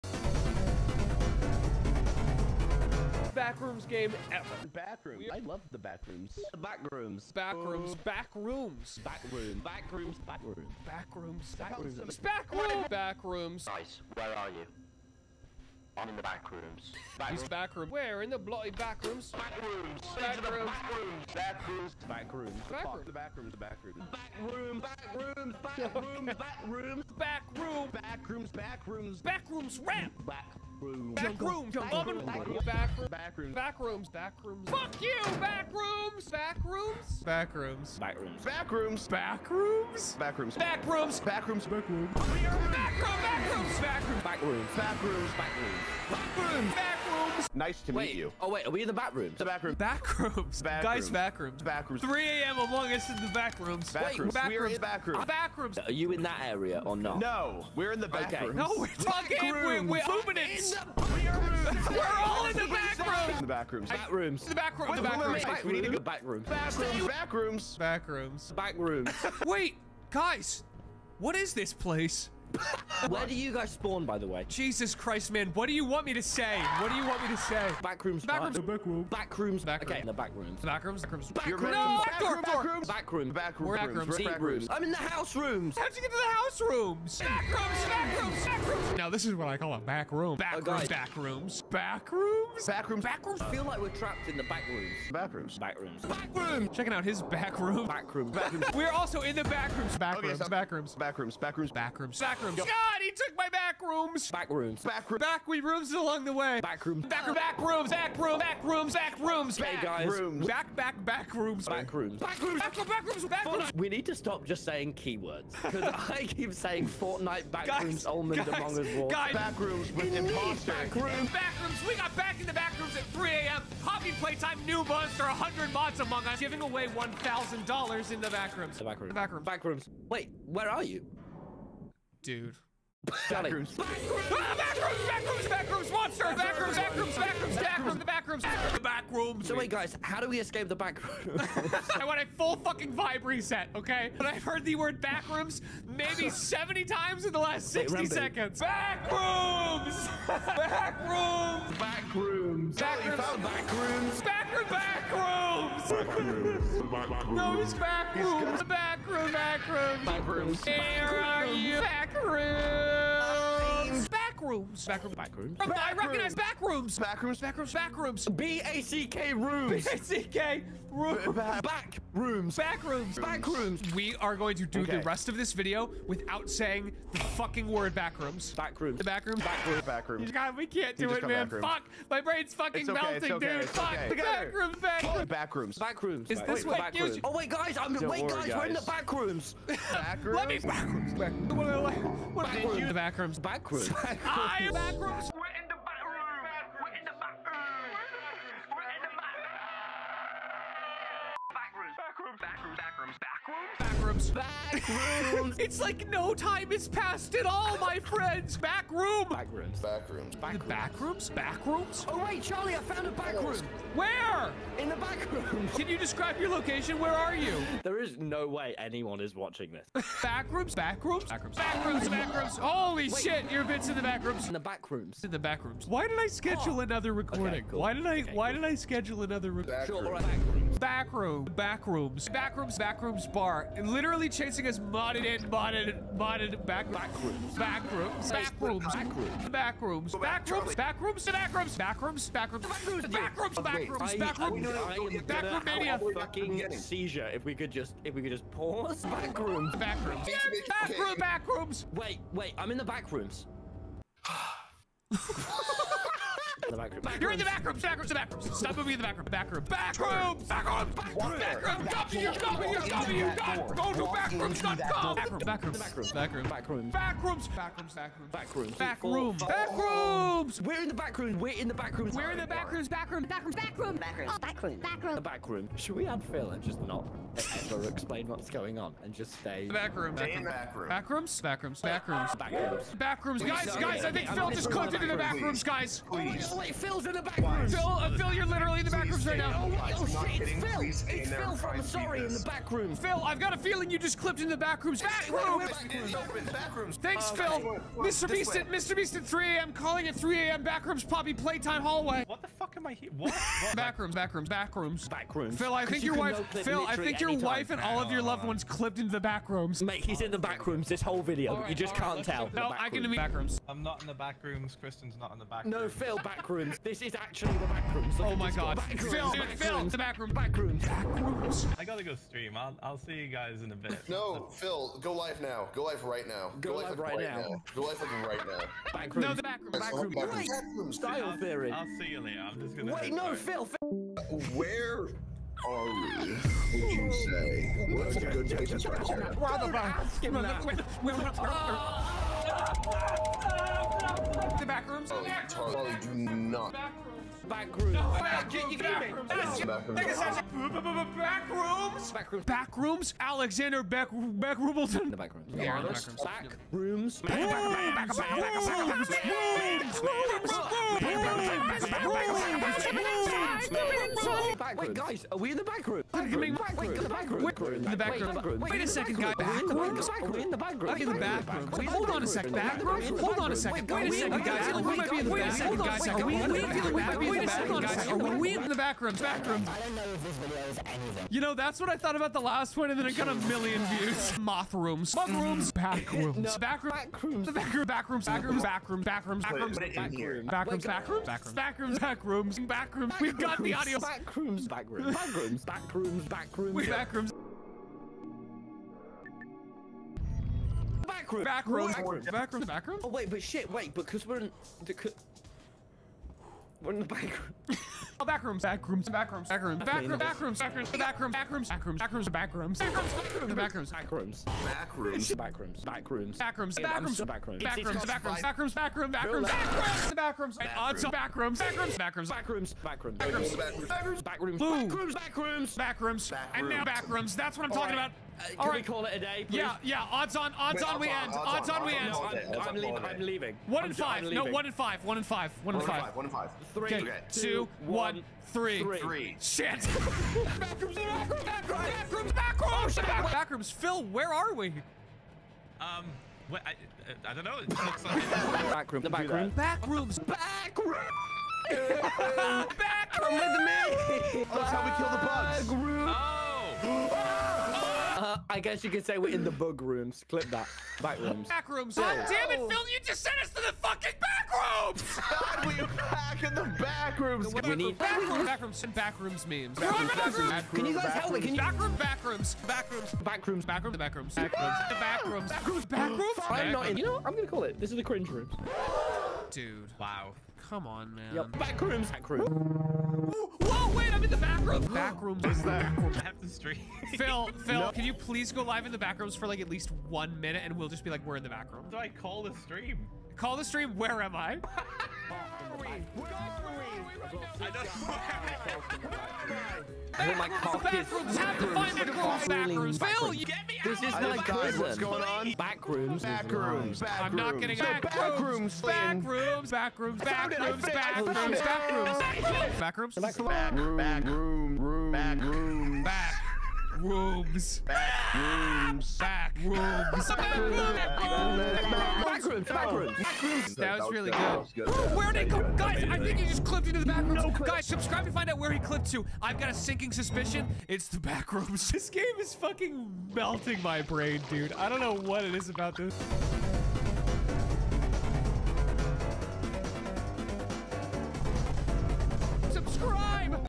Every Time Charlie, Tommy, and Ranboo say “Backrooms”
._. my brain melted after editing this- you're welcome.

✿ timestamps
0:00 We Made the Backrooms HILARIOUS...
0:37 We Made The Backrooms Even Funnier...
3:32 We Made The Backrooms DANGEROUSLY HILARIOUS...
7:38 Trapped in the Hardest Backrooms Levels...